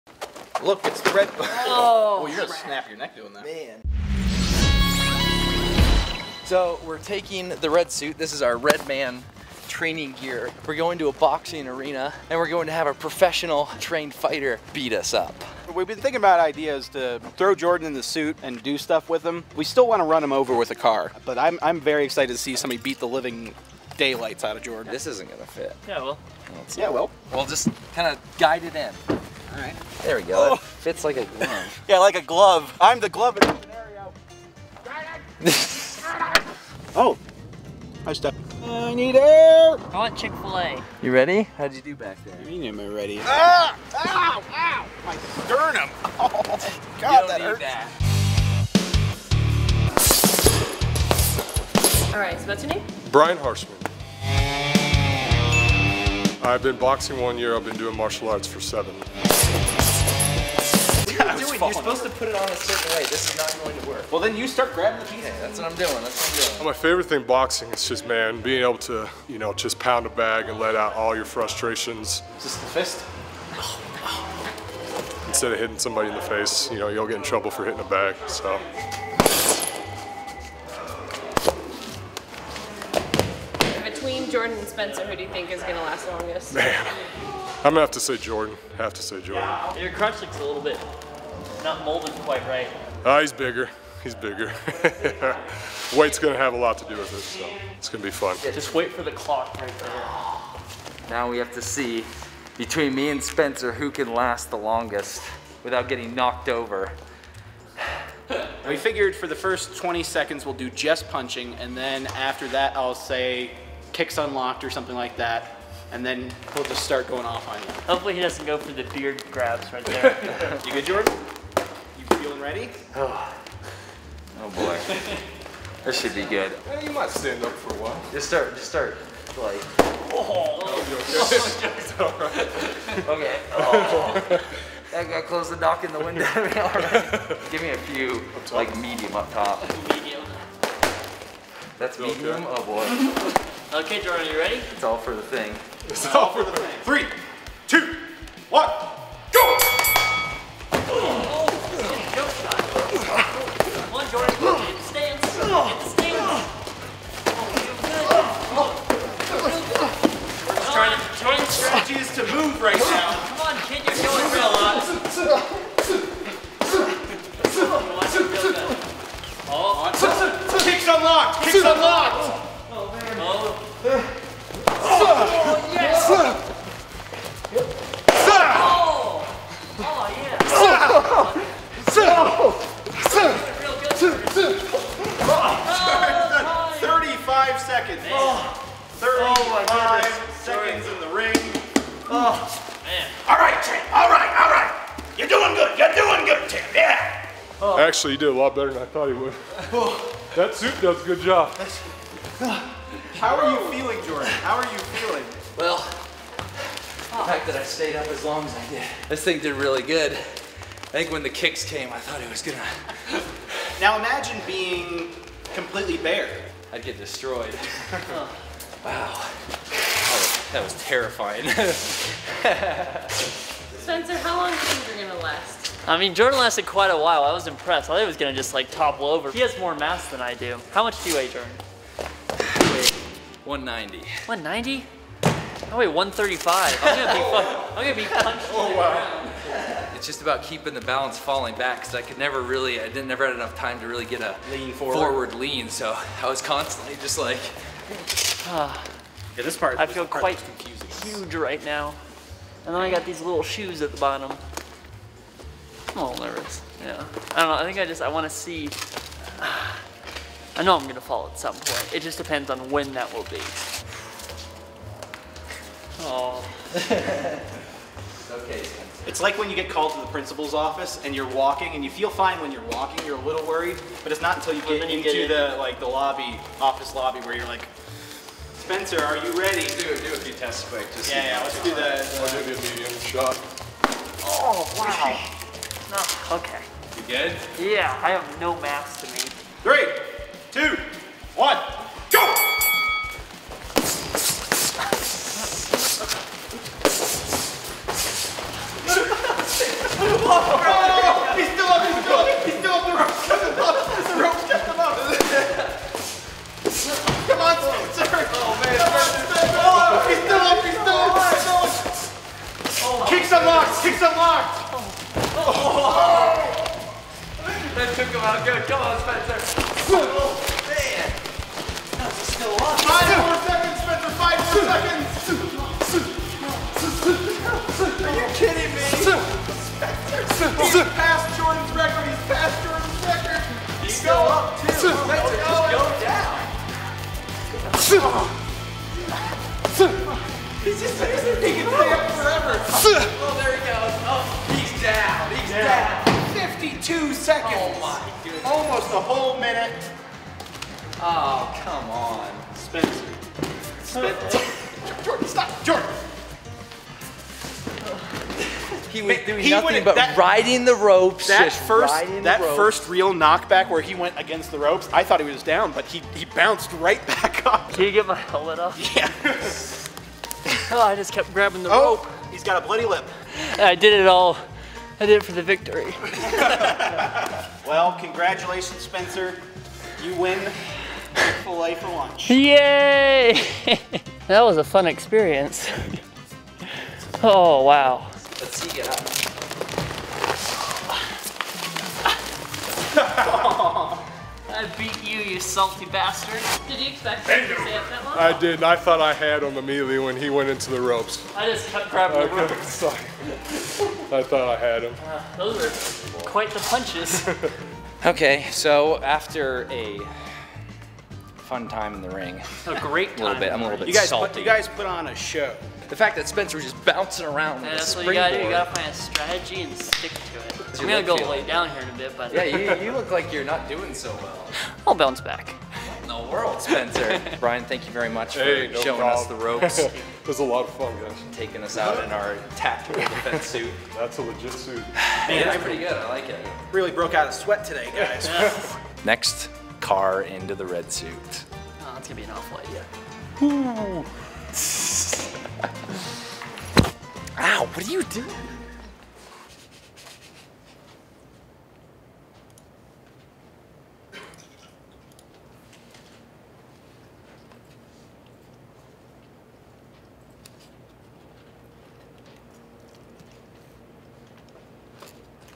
Red Suit VS Martial Arts Pro
Prepare for the pummeling of a lifetime!

The guys put the red suit to the test and decide to get in the ring with local Martial Arts Pro/Boxer Brian Harshman.